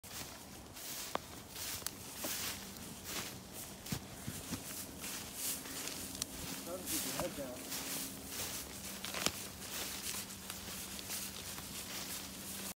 Headed to Walmart
I headed to walmart with some pals